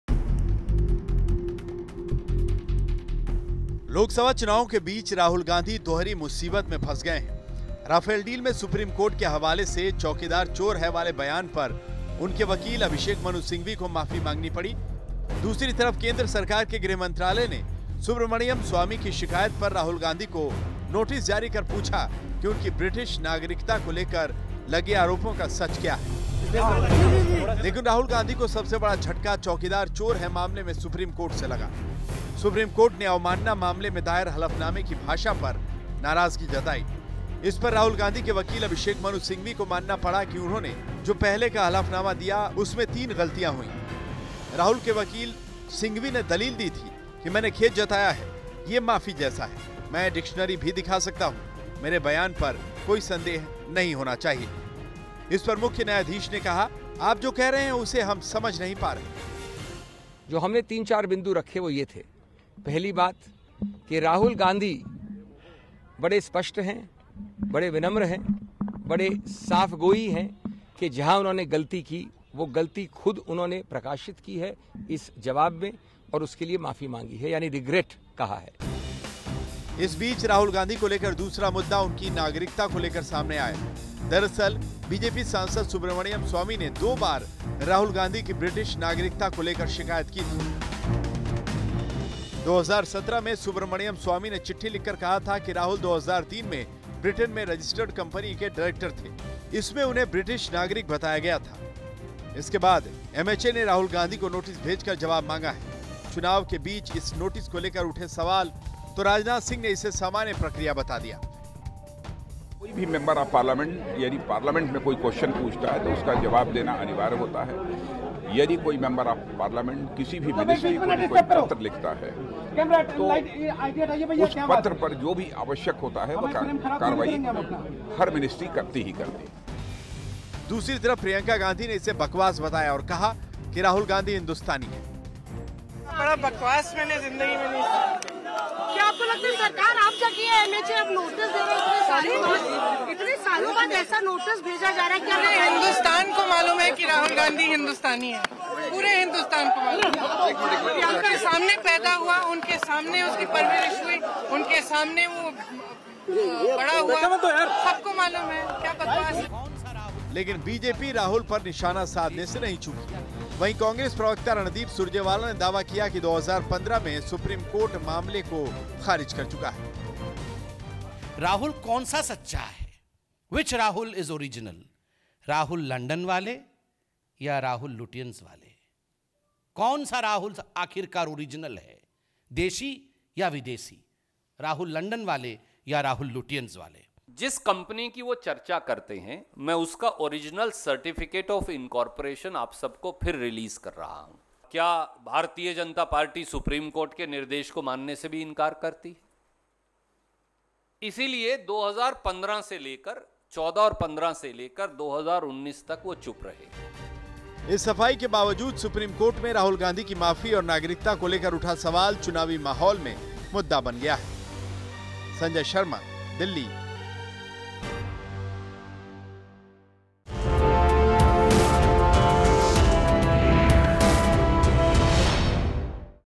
राहुल गांधी किस देश के 'वासी' हैं? | UP Tak
राहुल गांधी किस देश के वासी हैं? ये सवाल उस वक्त आ खड़ा हुआ है जब देश में चौथे दौर का मतदान हो चुका है। अमेठी में राहुल गांधी की नागरिकता का विवाद खत्म हो चुका था। और तभी गृह मंत्रालय ने राहुल गांधी से दोहरी नागरिकता पर नोटिस जारी कर सफाई मांग ली। चुनावी माहौल में इस नोटिस पर सियासत होना लाजमी है। लेकिन इसके साथ ही आज सुप्रीम कोर्ट में राहुल गांधी की माफी से भी एक नया मोड़ आ गया है। इन दोनों ही मुद्दों पर करेंगे चर्चा लेकिन पहले ये रिपोर्ट देखिए।